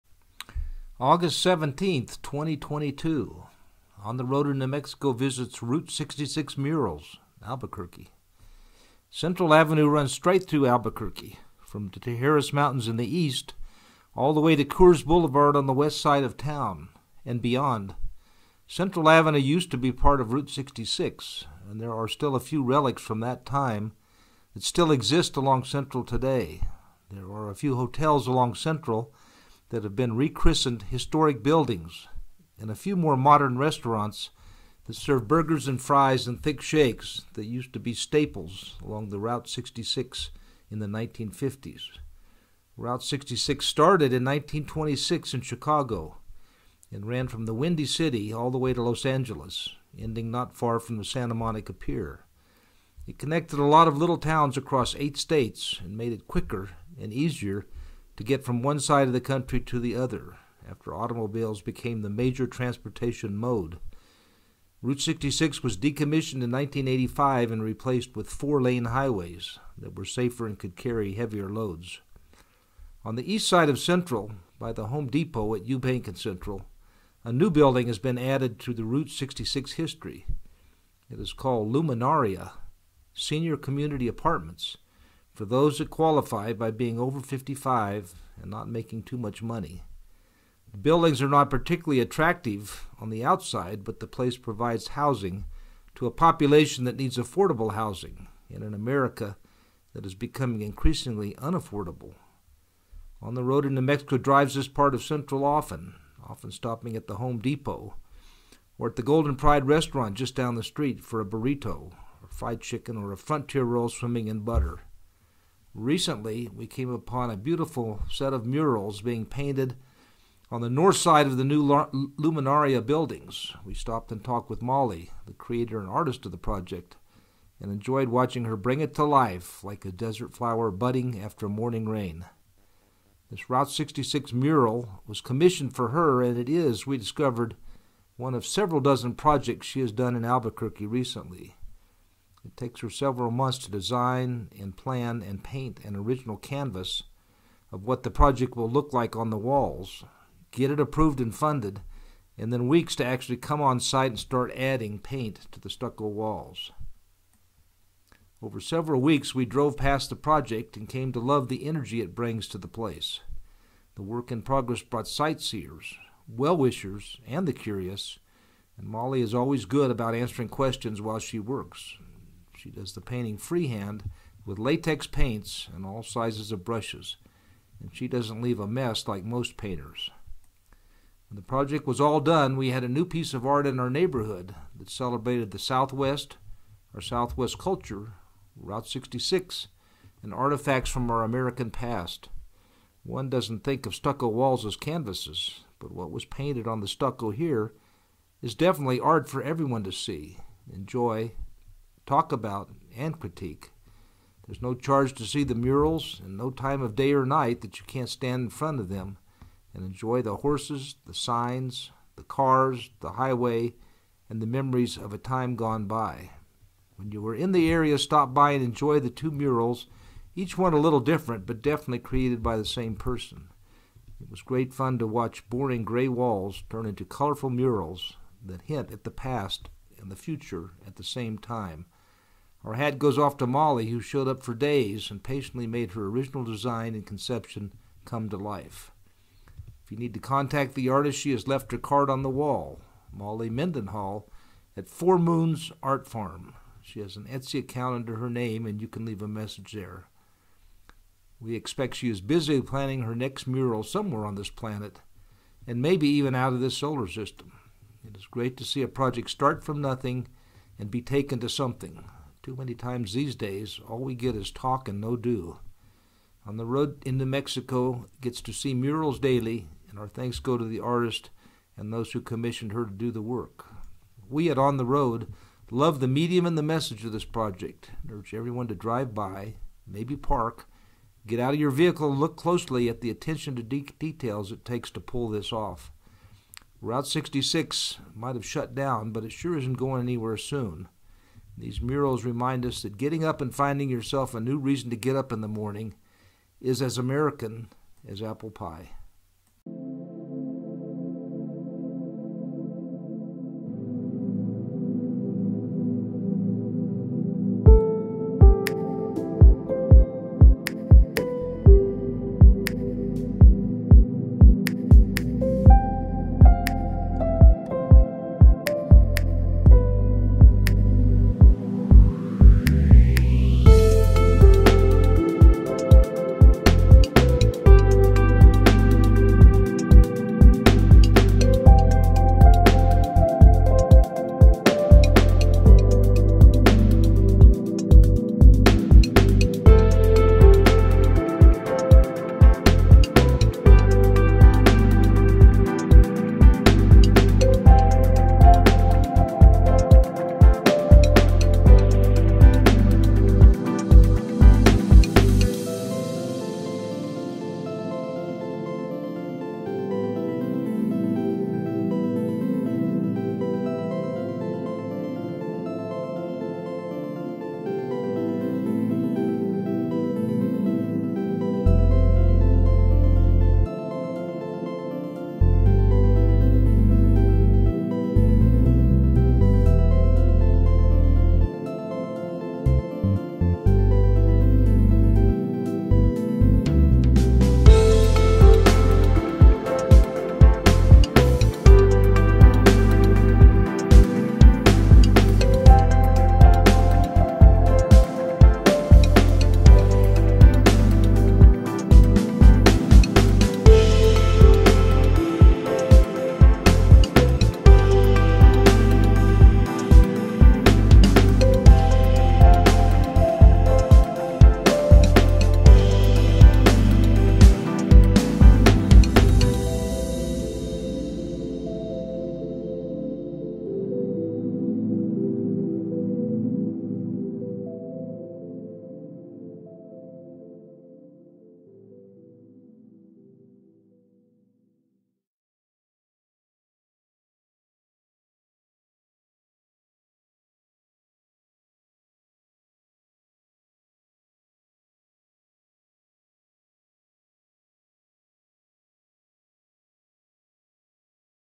On the Road in New Mexico Visits Route 66 murals, Albuquerque
The famous Route 66 used to run right through the middle of Albuquerque, all the way down Central Avenue. Even though the " Mother Road " succumbed to progress, pieces of it still remain. A couple of Route 66 murals recently went up on a building in Albuquerque by Eubank and Central. The artist, Molly Mendenhall, created the designs over months and took several weeks to complete her project. Now, On the Road in New Mexico, can enjoy art and history on the way to the Golden Pride restaurant for a Frontier Roll. Our right thumb goes up to Molly for bringing something colorful to our home city and reminding us that even cave men put art on their walls.